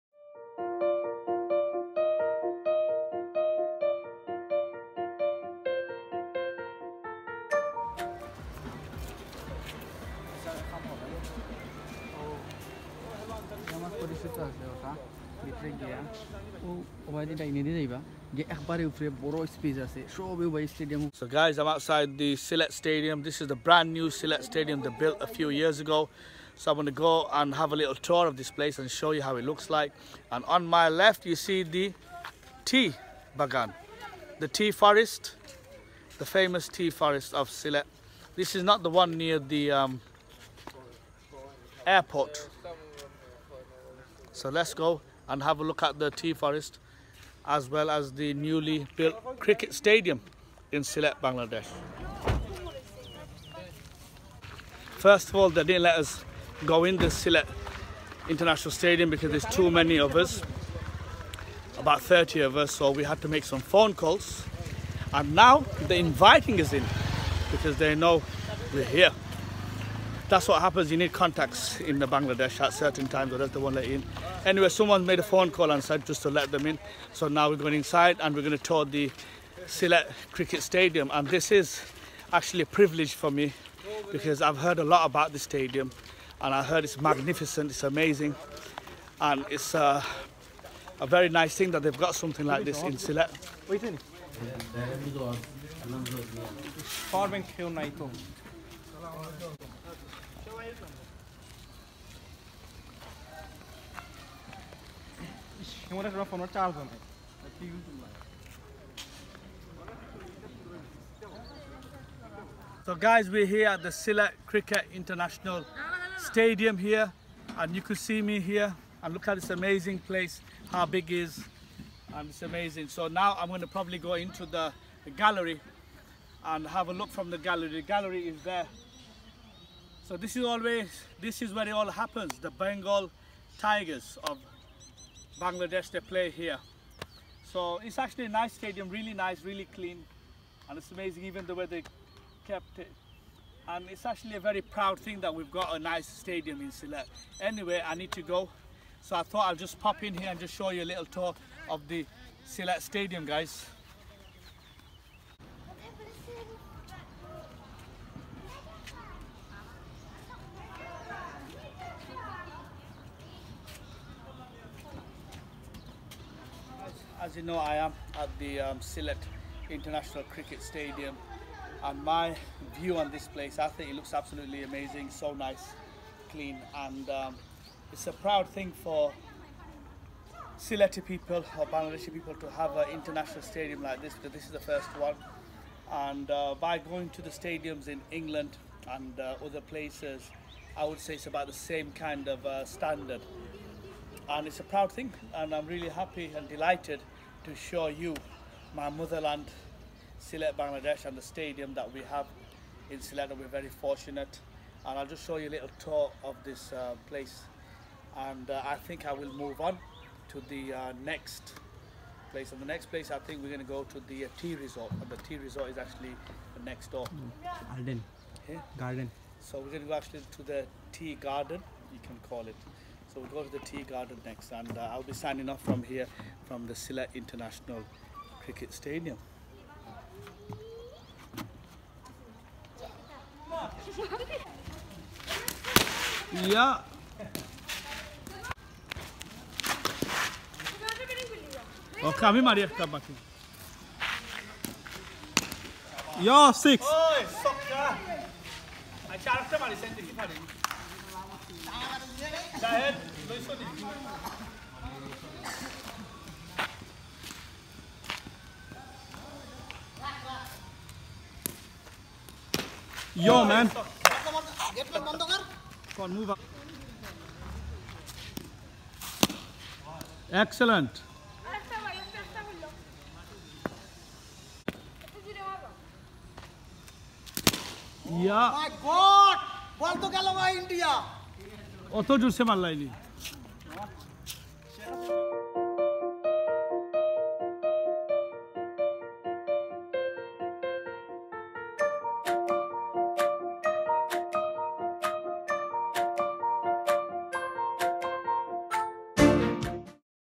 SYLHET CRICKET STADIUM BANGLADESH | Ash Vlogs
The stadium was expanded and renovated to host matches of the 2014 ICC World Twenty20. The renovation began in June 2013 and ended in mid-November 2013. The main pavilion building and the media centre have been constructed, floodlights have been installed, and seating arrangements were modified.[17][18] Another more striking part of the stadium is the newly built, country's first 'Green gallery'.[19]

The venue was again renovated in 2017 in order to upgrade the venue as an international standard stadium. A second tier was built in the east side gallery, increasing the seating capacity by 5000 and the green hillock was remodified.[20] As a result, the venue hosted the first leg of BPL 2017 matches.

It is the only Stadium in Bangladesh to have a Green Gallery. It also has Northern and Western Stand, Club House and Grand Stand. All this new expanded & renovated look of this stadium was designed by architect Masudur Rahman Khan. Executing & implementation authority: Bangladesh Sports Council. Requiring body: Bangladesh Cricket Board & Sylhet Divisional Sports Authority.